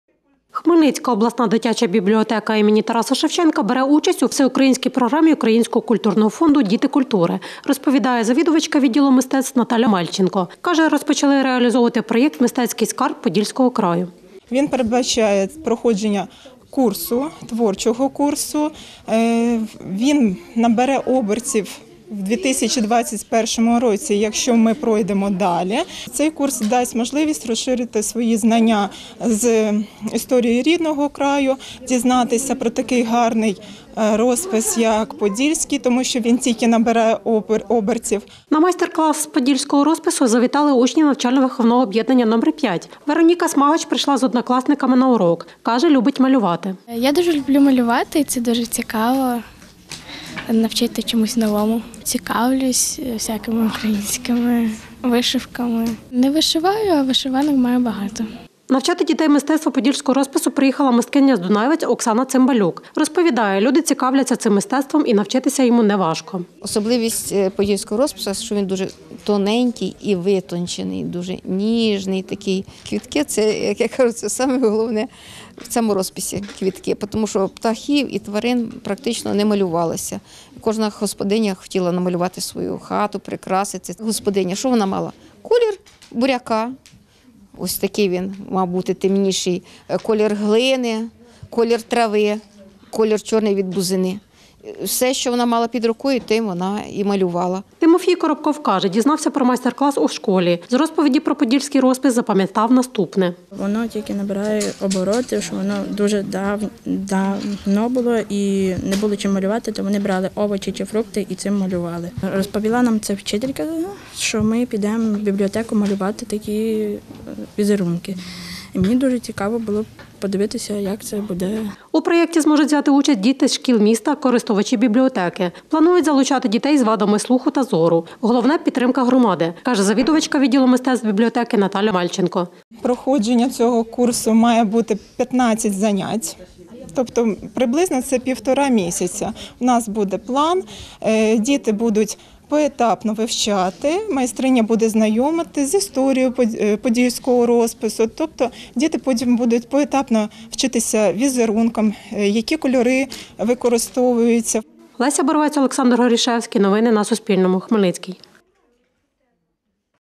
Хмельницьких школярів навчали мистецтву подільського розпису
Хмельницьких школярів навчали мистецтву подільського розпису. В обласній дитячій бібліотеці імені Тараса Шевченка для них провела майстер-клас художниця з Дунаєвець Оксана Цимбалюк. Про це розповіла завідувачка відділом мистецтв Наталя Мельченко.